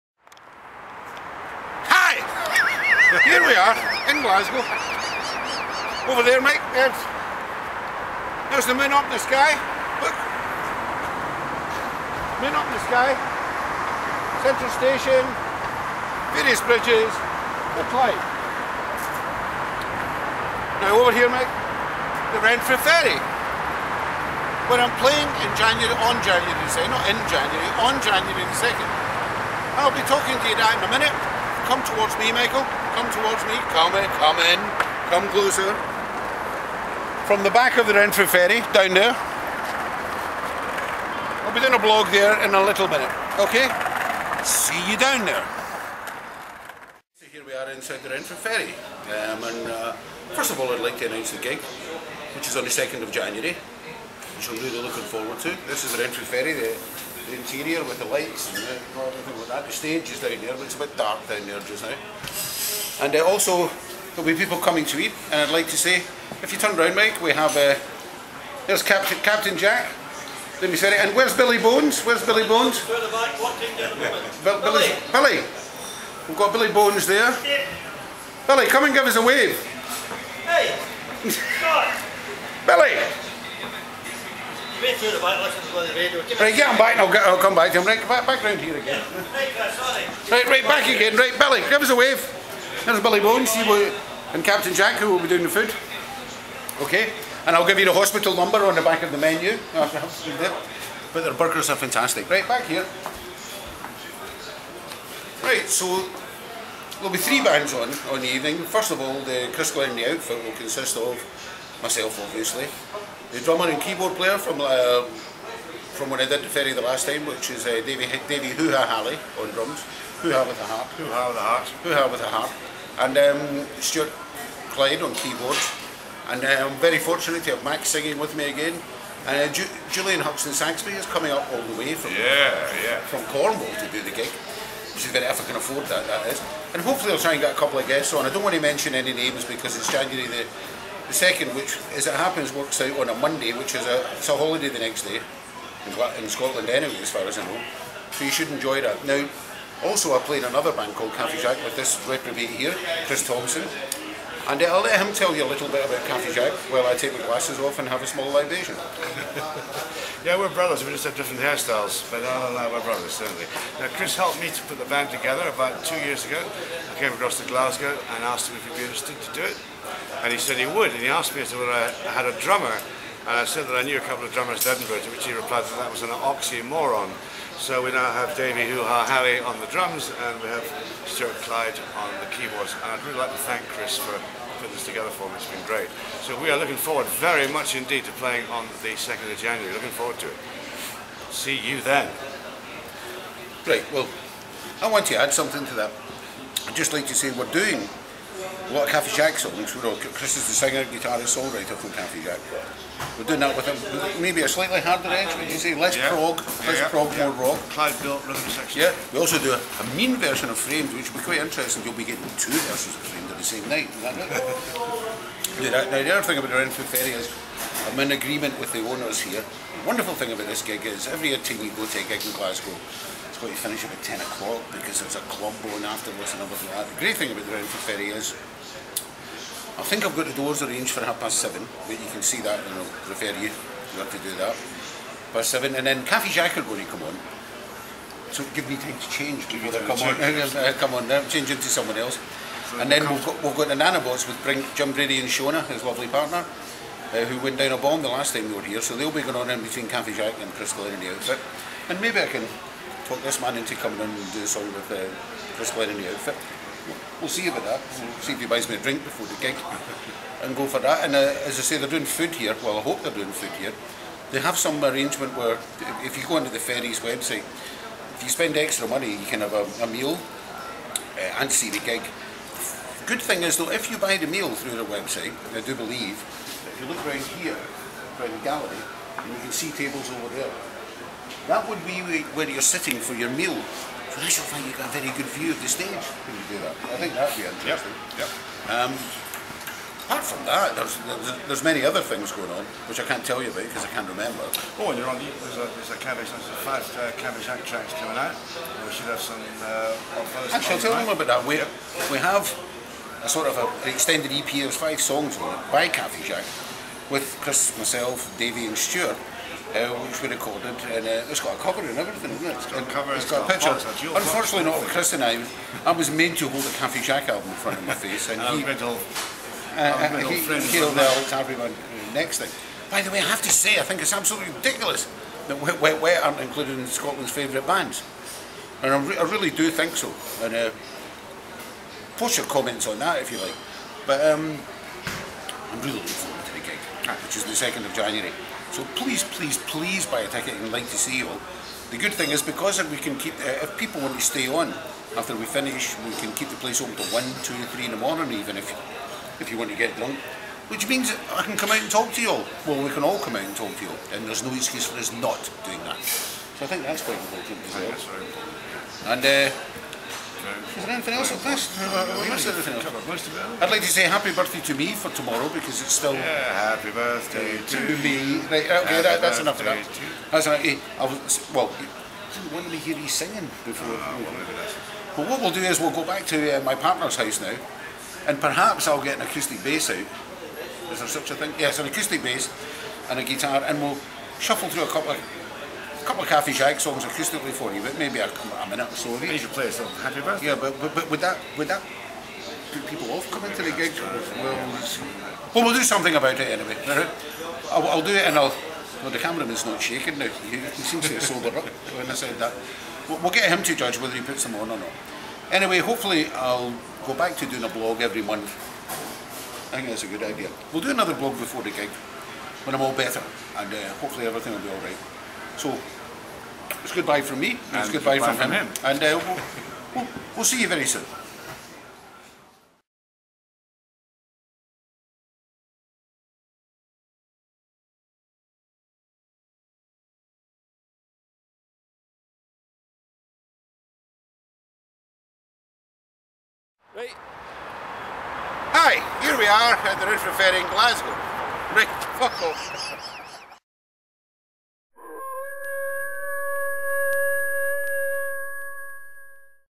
Chris Glen & Chris Thomson - Ferry Talk
CHRIS GLEN & CHRIS THOMSON explain about the upcoming gig at Glasgow's Ferry on the 2nd January...With CHRIS GLEN & The OUTFIT...CAFE JACQUES. & The NANOBOTS...Including from S.A.H.B.. MAX MAXWELL & JULIAN HUTSON SAXBY. A great way to kick off the new gig year!